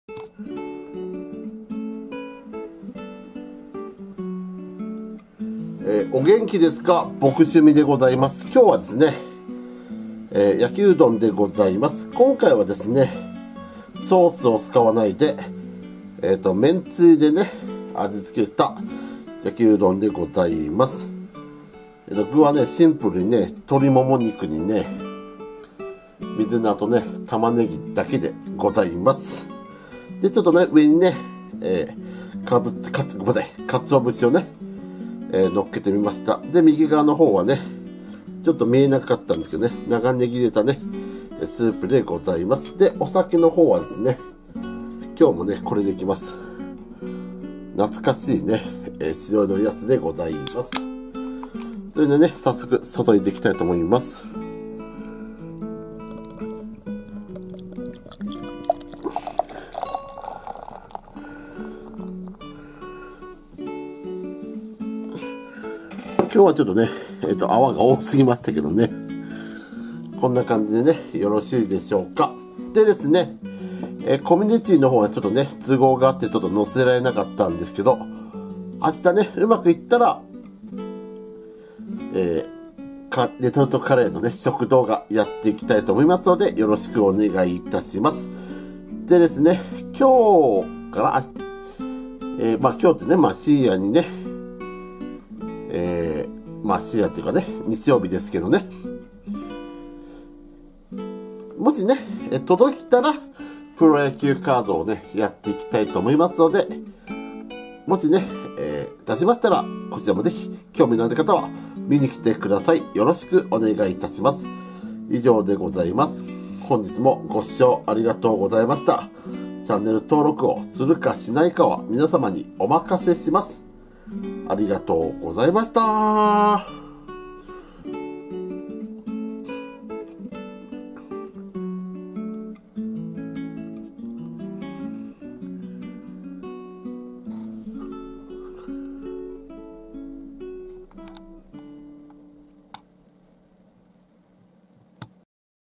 ２月18日の夕飯は焼うどんとスープです。（酒はウイスキーのスプライト割です🥃）
（主な材料）
うどん、人参、水菜、玉ねぎ、ウェイパー、長葱、めんつゆ